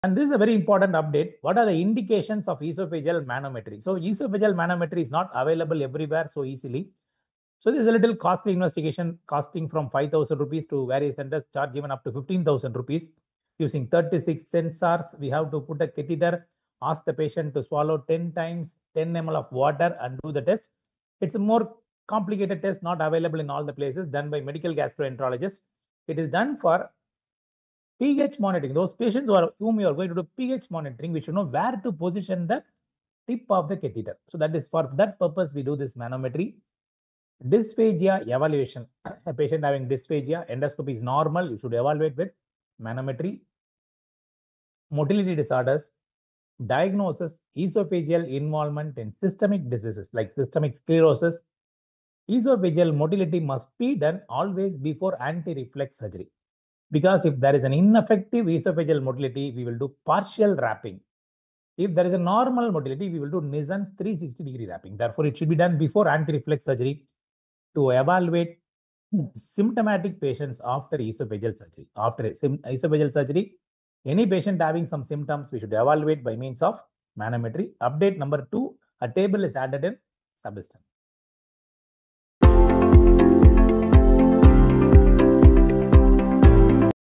NEET SS Surgical Prep | Sabiston 22nd Edition Updates – Esophagus & Stomach
We’re bringing you Update 2 from the Esophagus & Stomach section of Sabiston 22nd Edition!

🔔 Follow us for daily surgical updates, tips, and high-yield insights directly aligned with NEET SS 2025.

💡 What you’ll learn in this video:
Essential surgical concepts of the esophagus and stomach
High-yield points from Sabiston 22nd Edition
Practical insights for exam and clinical application

Watch the full Sabiston surgical updates series on DocTutorials Super Speciality youtube channel  and stay ahead in your NEET SS prep!